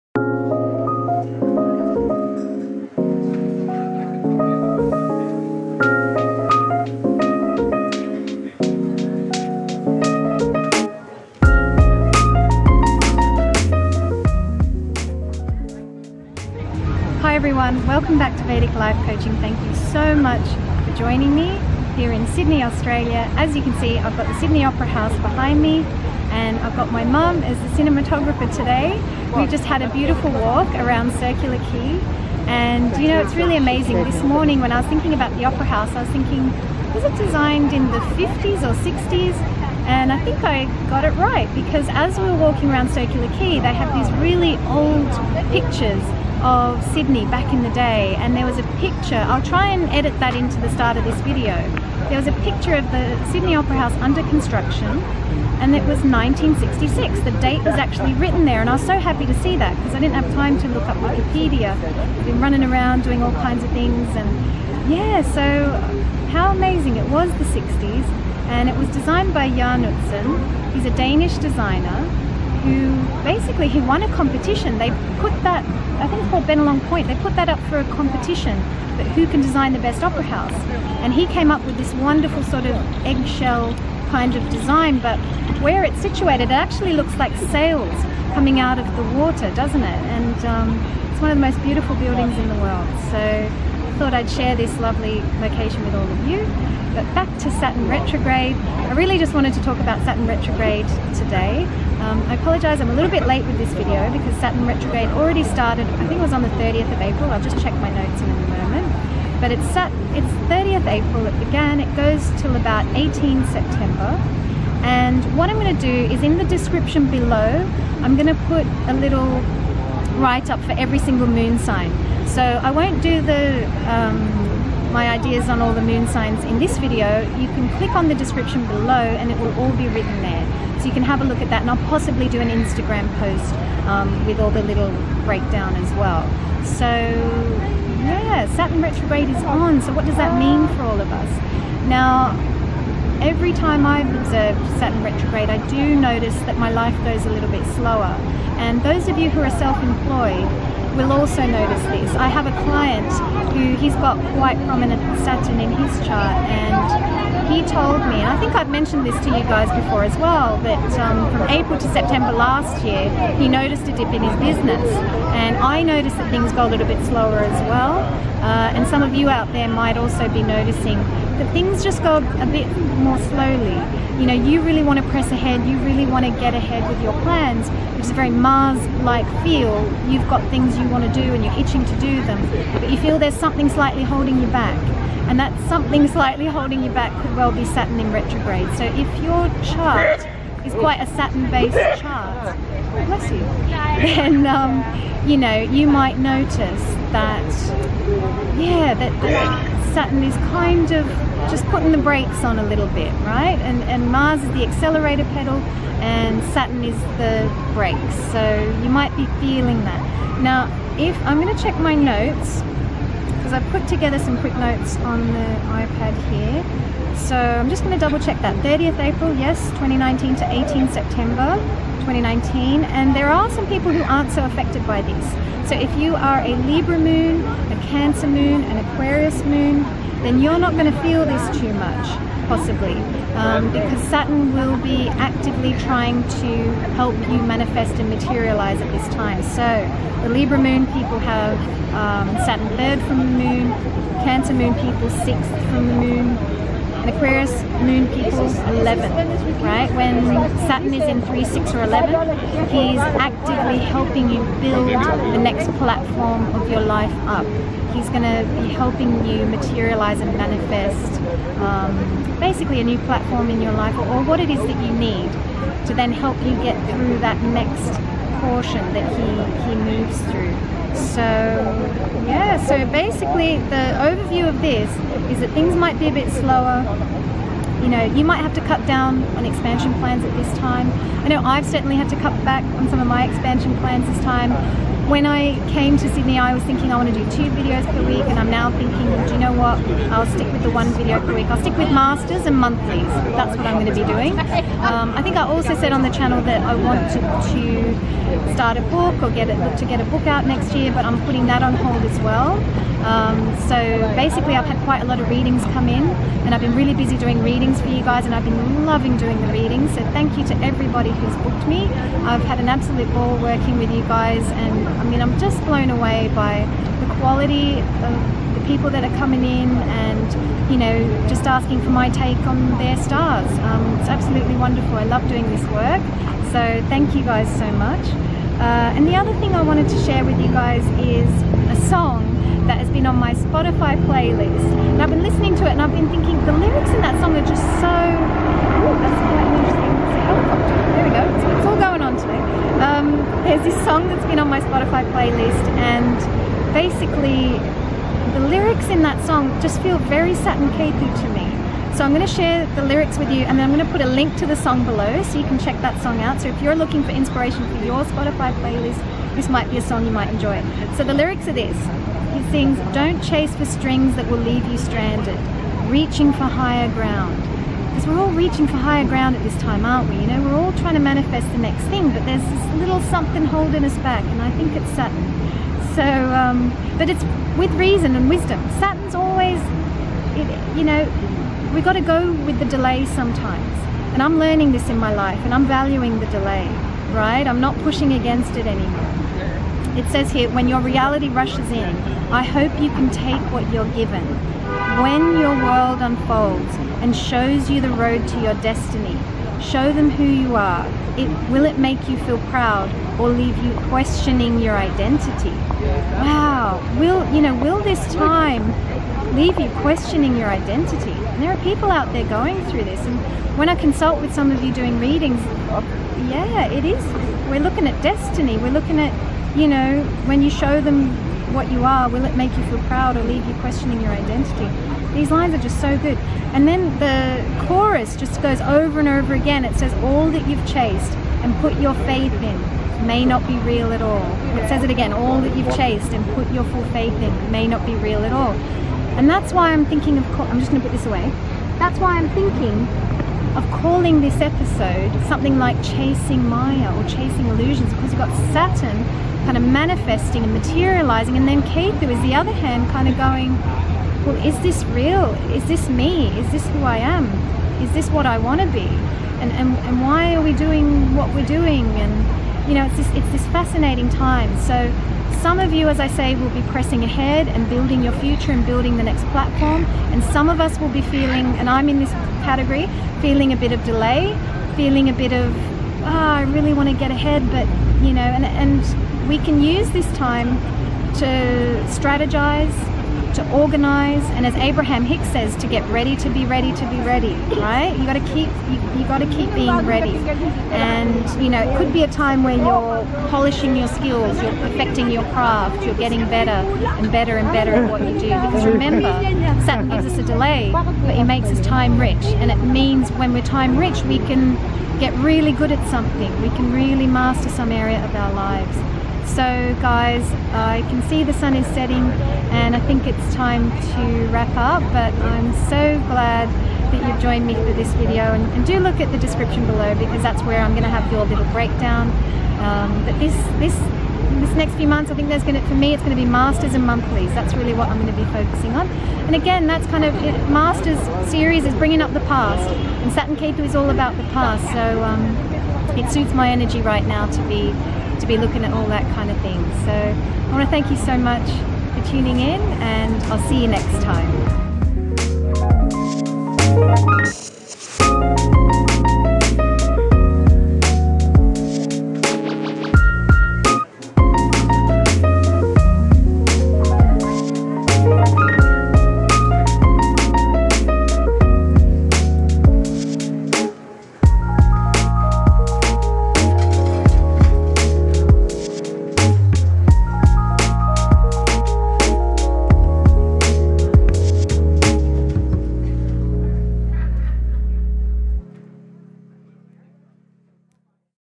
Saturn Retrograde 2019 April to September // Chasing Maya
Through working with all of you, I've discovered that many people are experiencing the following during this Saturn/Ketu conjunction: delays, feels like something's holding you back slightly, motivation to do something but then time vanishes. Now that Ketu is quite tightly conjunct Saturn, this isn't a typical Saturn retrograde – not only are we experiencing all the usual Saturn Retro symptoms, but we're also questioning our lives on deeper levels – who am I, what's my identity, what do I do everyday, is this real, is this an illusion, why am I stuck in the past, why is the past still repeating, why can't I break through to higher ground and so on.

Studying astrology, or anything spiritual, is a wonderful remedy at this time (for Ketu) as is service to anyone around you who is in more need than you are (for Saturn).

Written mini-reports ~ to appear on insta @vediclifecoach

ARIES moon: 9th house. Any delays at this time could serve you, in that this is a great time to review deeply held beliefs about your self and your life.

TAURUS moon: 8th house. Career and cashflow – both could be slowed a bit, but this isn’t a problem, Saturn’s getting you ready for better times in both areas, hang in there.

GEMINI moon: 7th house. Good time to rework health routines. Realise that your relationship or progress at work/business may feel stalled a bit at times.

CANCER moon: 6th house. Work with Saturn to materialise new opportunities at work or even a new job, and rest to preserve your health whenever you can.

LEO moon: 5th house. Might be harder to concentrate over the coming months, and romance may feel foggy at times. Keep on keeping on, good times are ahead for you.

VIRGO moon: 4th house. Great time to work on creating better fitness routines and reworking your diet too. Also a great time to rework your house/where & how you live.

LIBRA moon: 3rd house. Business expansion, a boost to confidence – keep working with Saturn and go easy on younger siblings.

SCORPIO moon: 2nd house. Gratitude for the simplest of things will be like investing in a karmic account you can cash-in later. Great time to rest and work on your health.

SAGITTARIUS moon: 1st house. Hang in there Sag Moon, you are being polished and made new, Saturn will reward you big for this when he is Third from your Moon.

CAPRICORN moon: 12th house. Every good effort you make during Sade Sati will be noted and rewarded – all good karma you will cash-in when Sade Sati is done, hang in there dear Capricorn Moon.

AQUARIUS moon: 11th house. Keep building, keep taking advantage of this time. If you noticed a slight dip in business, this could be why, but keep at it, Saturn’s on your side.

PISCES moon: 10th house. Unexpected expenses could appear, plus you may need to work a bit more diligently at work. Keep your head down, stay positive, Saturn will be rewarding you next year.

Use you freewill to boost compromised energies

VIDEO CREDITS

And my mum for being behind the camera!